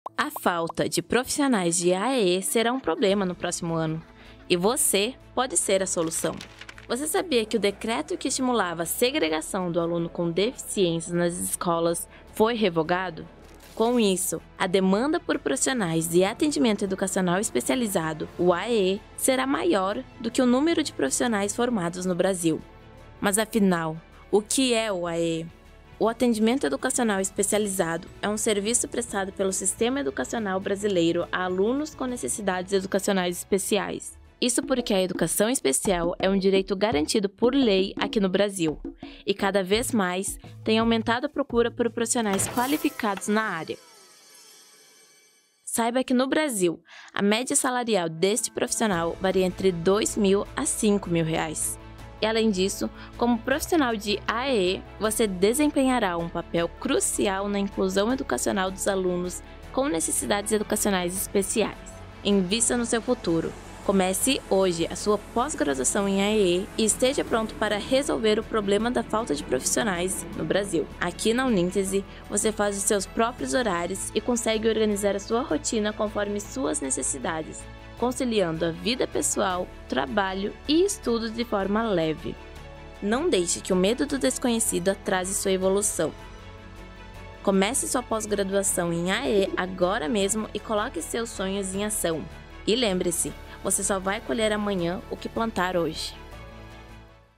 Um profissional requisitado no mercado | Atendimento Educacional Especializado
PROCURA-SE POR PROFISSIONAIS QUE DESEJAM PROMOVER A INCLUSÃO!

⚠️ A carência por profissionais de AEE é muito grande. Há vagas, mas falta especialistas na área!

Seja você a SOLUÇÃO, e desenvolva as suas habilidades e competências para promover estratégias que nortearam a inclusão de alunos com necessidades educacionais especiais nos espaços escolares.